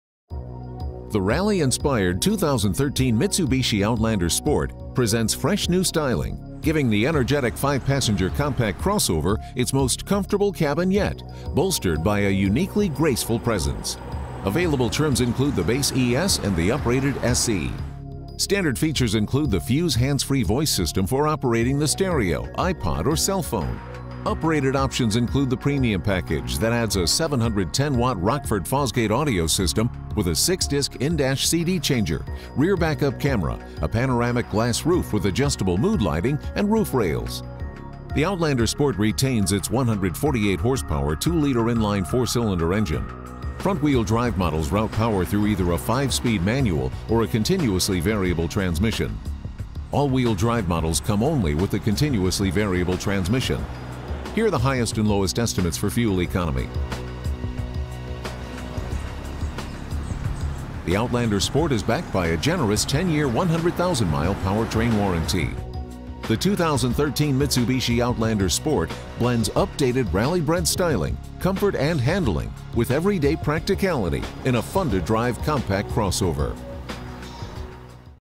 2013 Mitsubishi Outlander Sport Overview
The 2013 Mitsubishi Outlander Sport blends fresh new styling with everyday practicality in a fun-to-drive compact crossover.